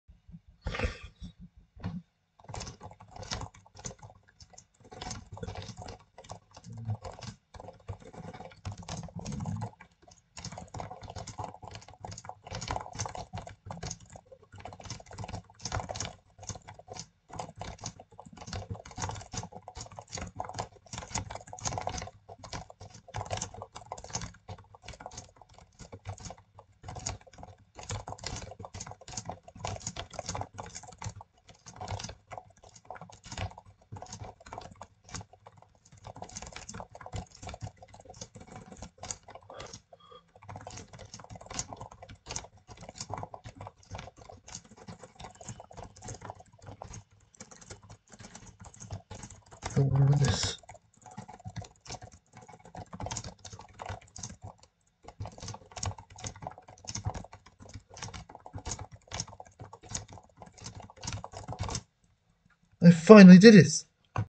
I got 115 words per minute, one of my best runs for 2024.
I had gotten a good word rate of 115, but needed some time to improve.